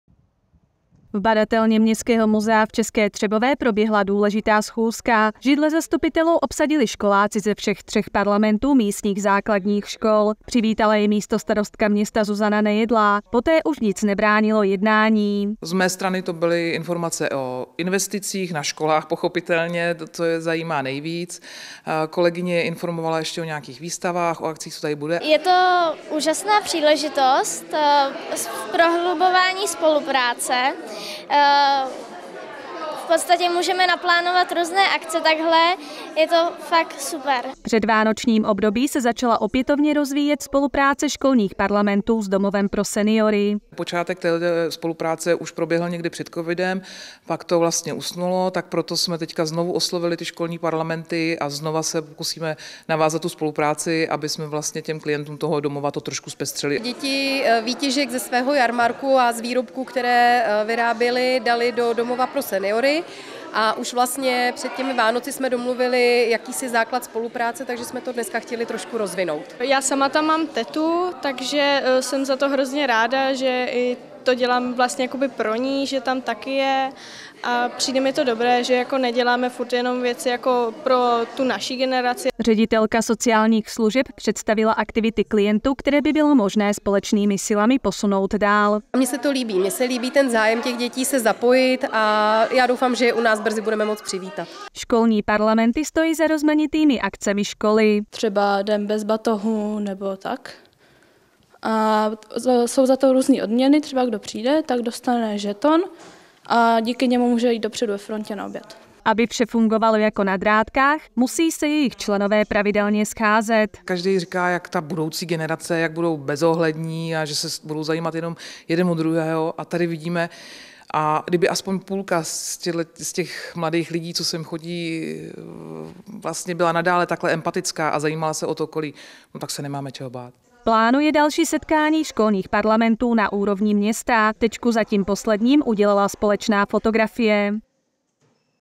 Česká Třebová: Školní parlamenty se sešly s místostarostkou Zuzanou Nejedlou
Školní parlamenty všech třech českotřebovských základních škol se vydaly do městského muzea. V badatelně, kde probíhají jednání městského zastupitelstva, se sešly zejména s místostarostkou Zuzanou Nejedlou. Školáky zajímali plánované investice ve školách. Na programu bylo i prohloubení spolupráce s Domovem pro seniory.